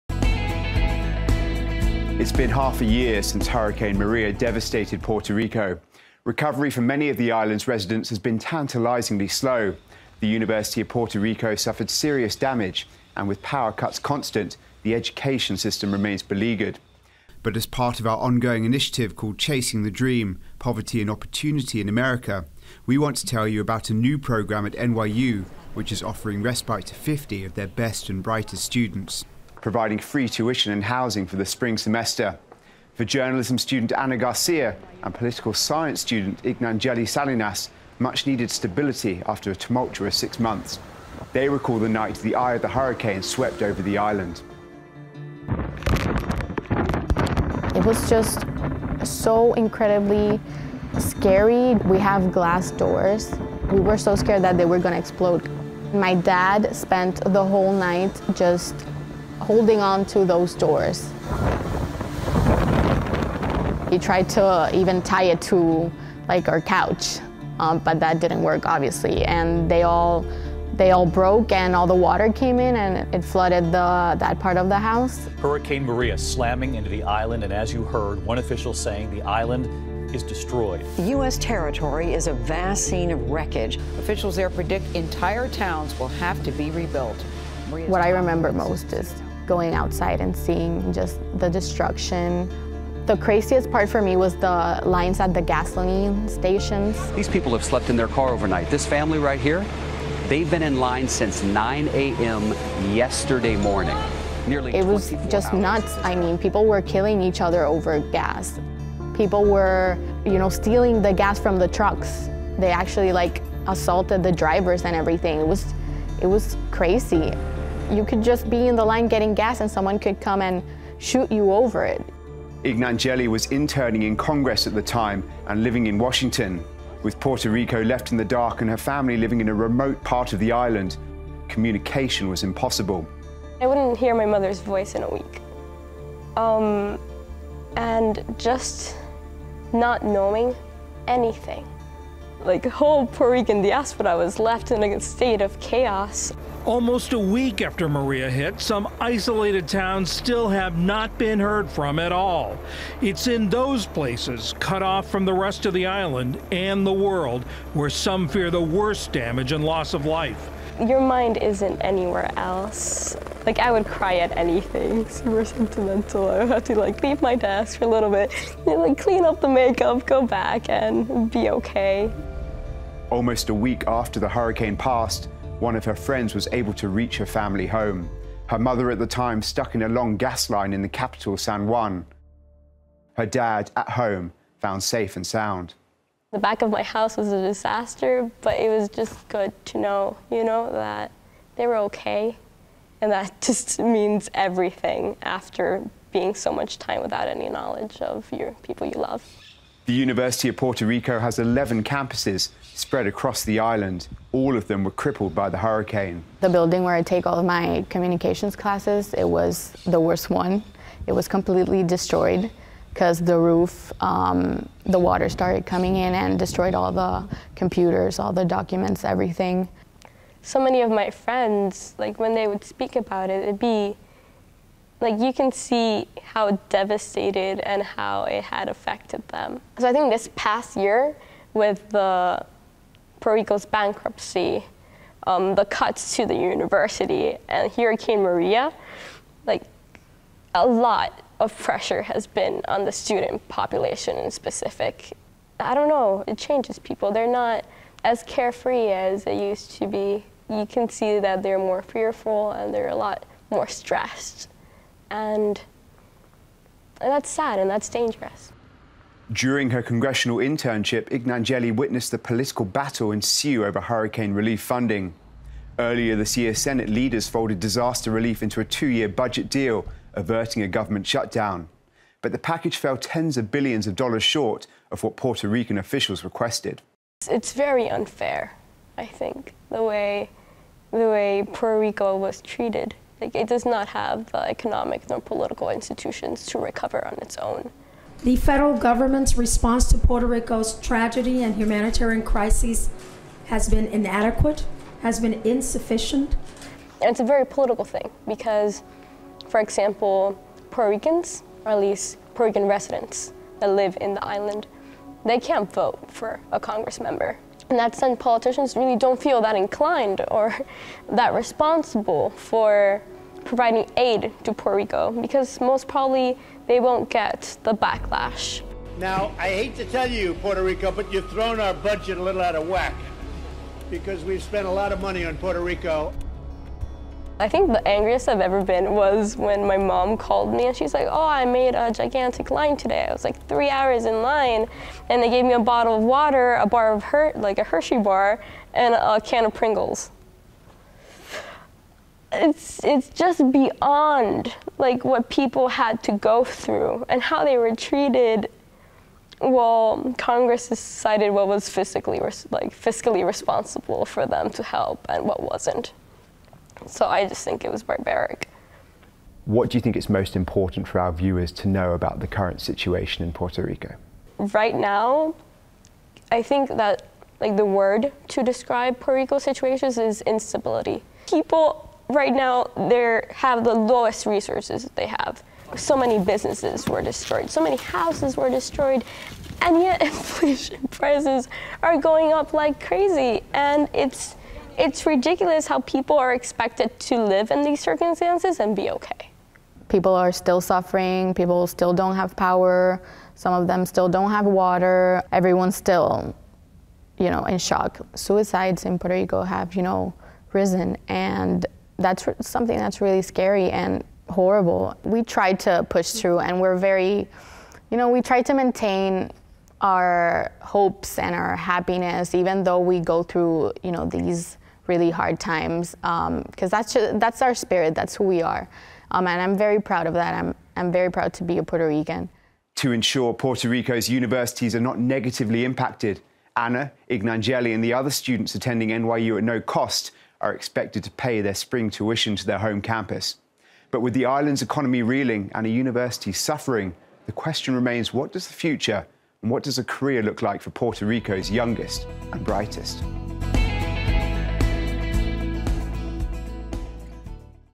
After Maria, An Uncertain Future
The University of Puerto Rico was devastated by Hurricane Maria. All eleven of their campuses were closed down in the weeks following. With the island’s education system in a state of flux, 50 of their students, including Ana Garcia and Ignangeli Salinas, were offered the chance to rebuild their education at NYU this spring. We take a look at these students’ journeys in the program as part of our multi-platform reporting initiative “Chasing the Dream: Poverty and Opportunity in America.”